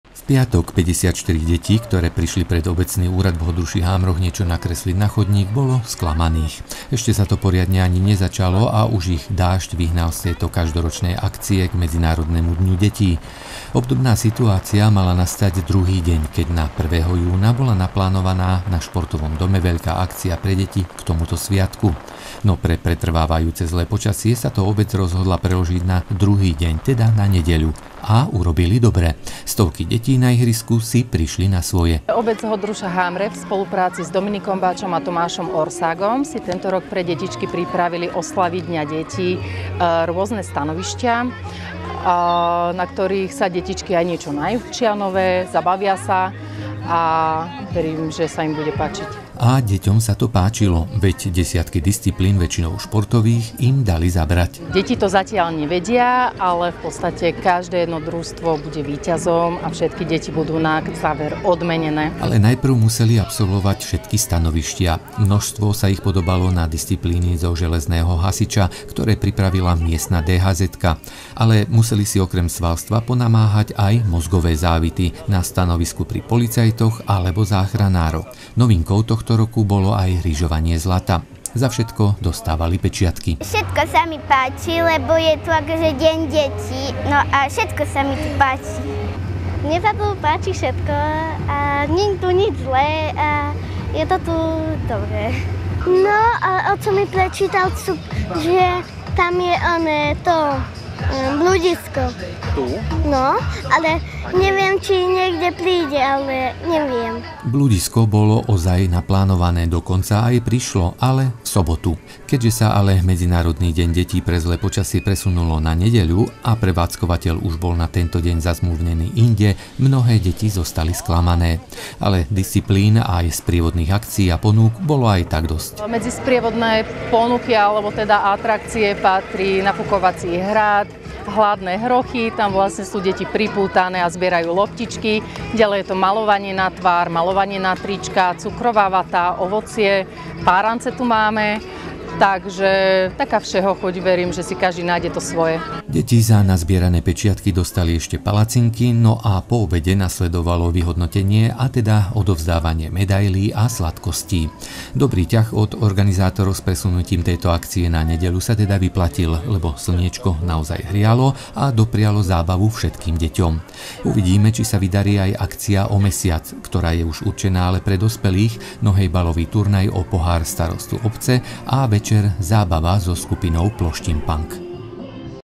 Aj deti ryžovali zlato
Reportáž z kreslenia na chodník a MDD v Hodruši - Hámroch.